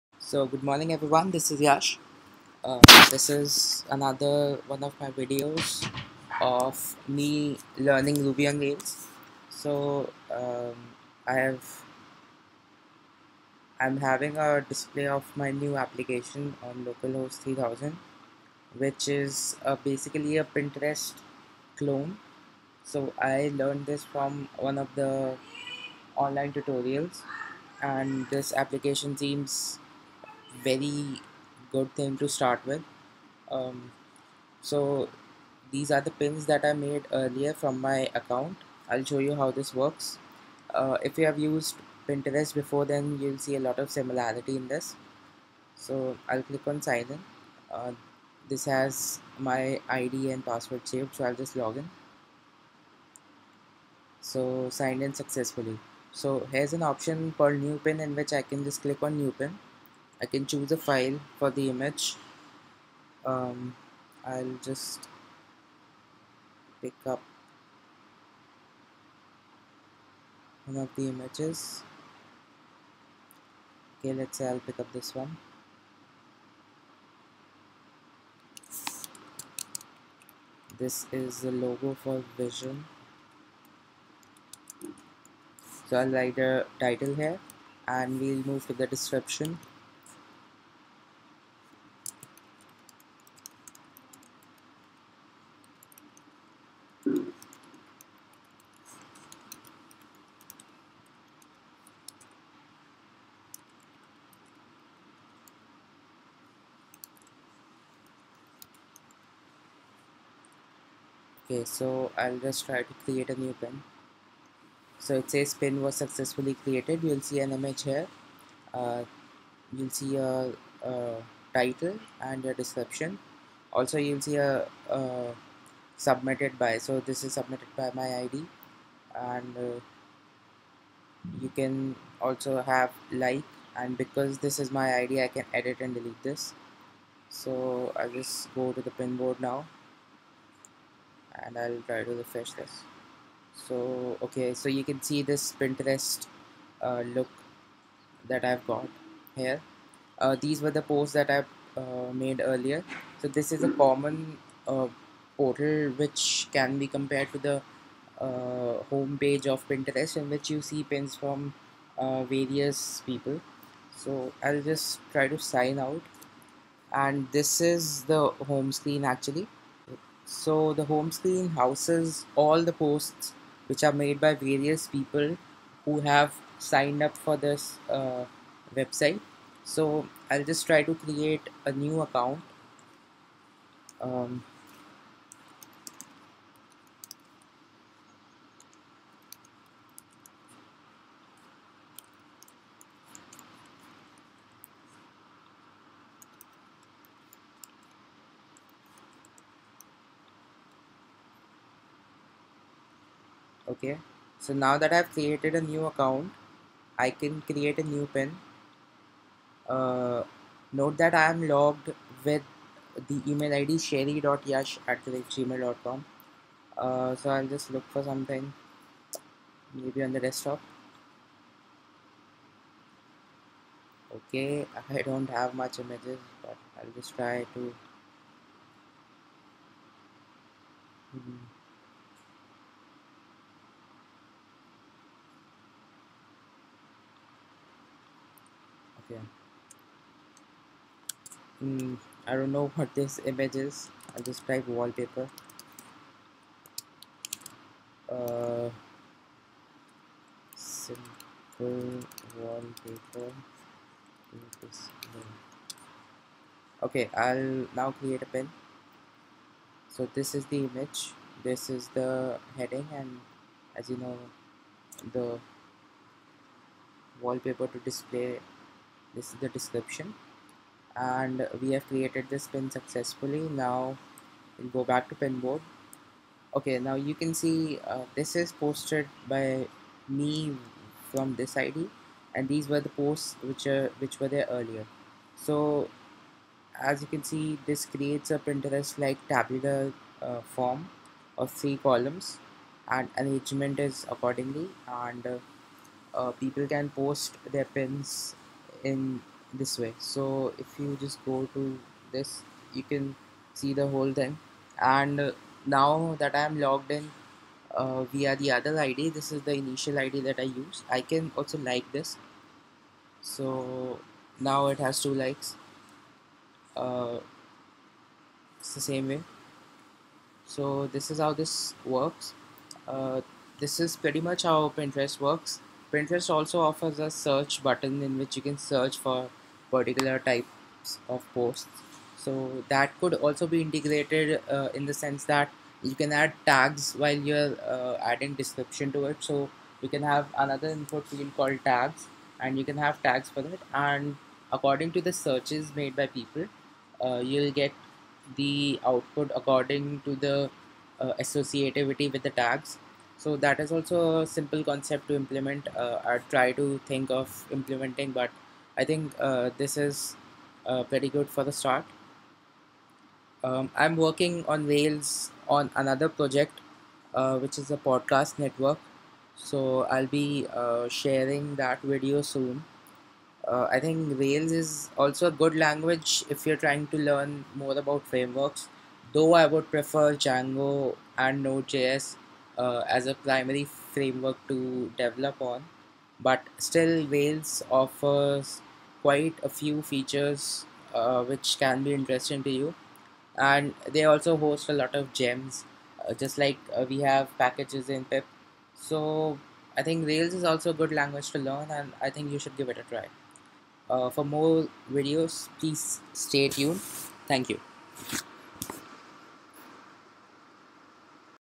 PinHack Ruby on Rails
This is a Pinterest like application which allows you to sign in, post your pins, edit them and also like others' pins. The layout is just like Pinterest , tabular with 3 column arrangement. Built on Rails.